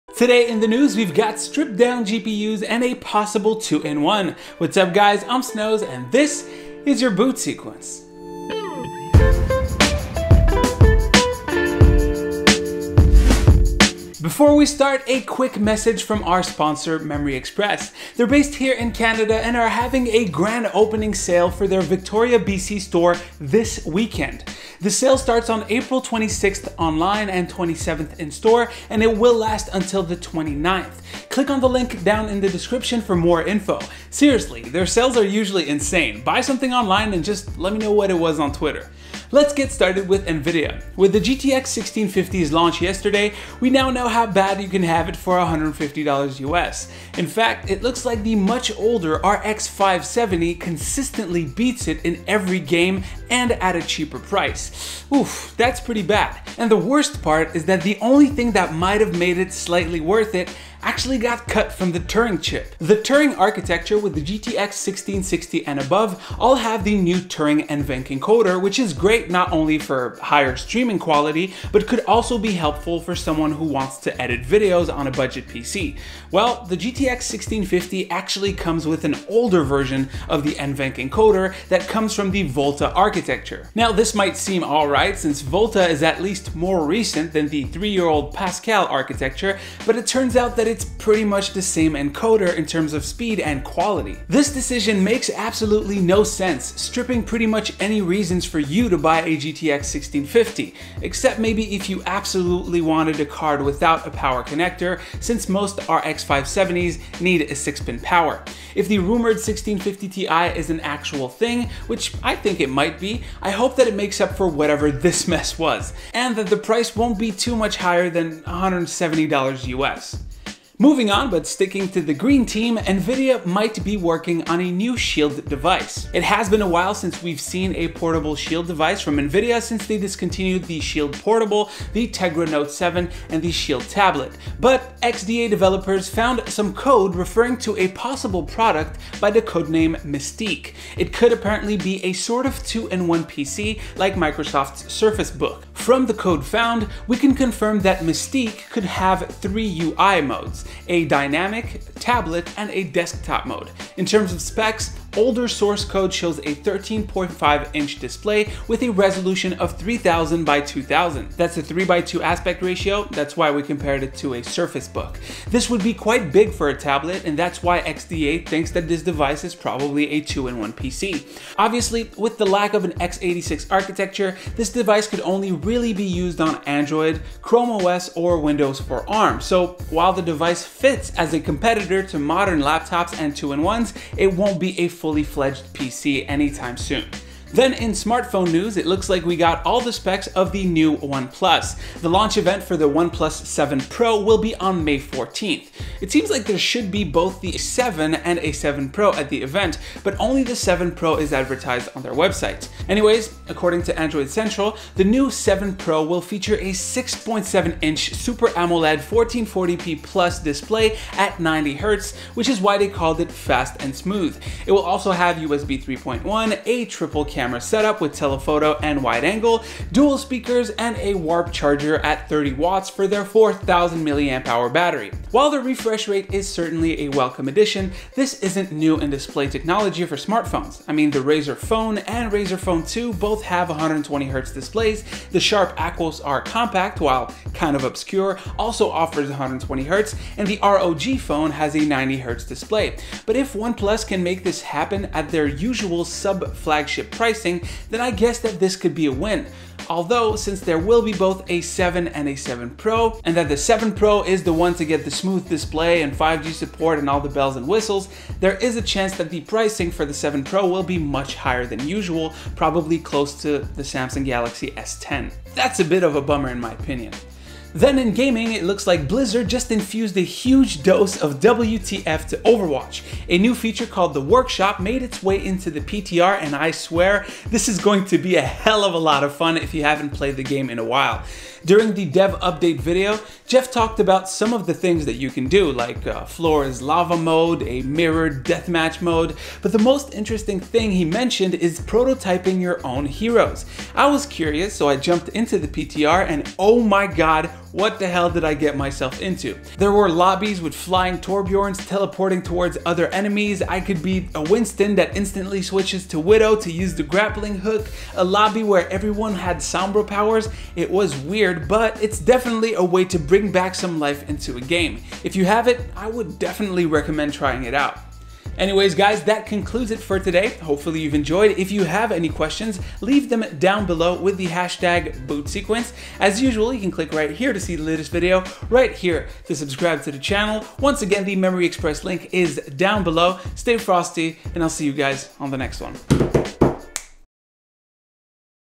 Nvidia GTX 1650 Has an Old Encoder!
With the GTX 1650’s launch yesterday , we now know how bad you can have it for 150$ US. In fact, it looks like the much older RX 570 consistently beats it every game and at a cheaper price ! Oof , that is pretty bad.  And the worst part, the only thing that might have made it slightly worth it actually got cut from its chip.

The turing architecture with the GTX 1660 and above all have the new Turing Nvenc encoder which is great not only for higher streaming quality , but could also be helpful for someone who wants to edit videos on a budget PC. Well , the GTX 1650 actually comes with an Older version of the Nvenc encoder that comes from the Volta architecture. Now this might seem alright since volta is at least more recent than the 3 year old Pascal but it turns out that it's pretty much the same encoder in terms of speed and quality.

TODAY'S DOSE OF TECH NEWS SOURCES